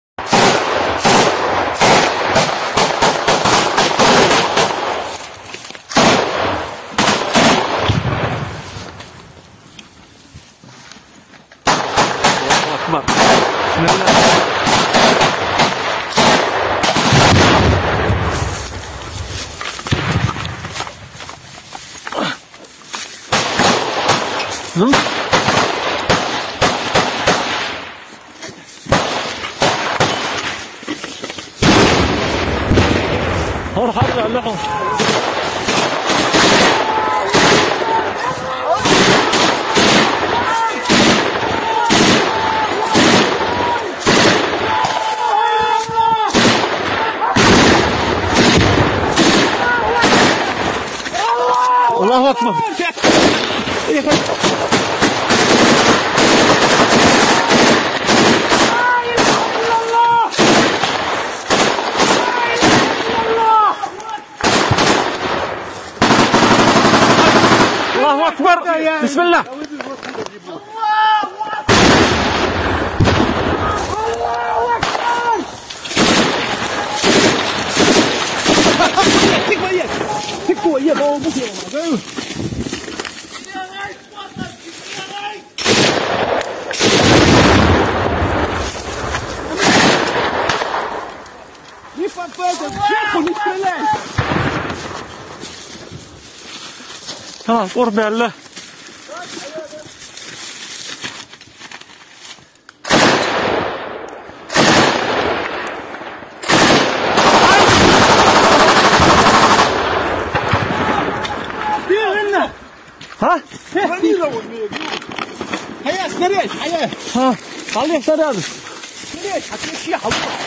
BATALION SHEYKHA MANSURA PRO UKRAINA
Ukraina 🇺🇦 vs Rusia 🇷🇺

Video Aksi Pasukan Pemberontak Muslim Chechnya anti Rusia - Pro-Ukraina "Battalion Sheykha Mansura" ikut menyerang dan menghancurkan militer Rusia. Sedangkan Pasukan Muslim Chechnya Pro-Rusia "Battalion Kadirov", mengalami kekalahan telak di Ukraina. @ethanwmf8603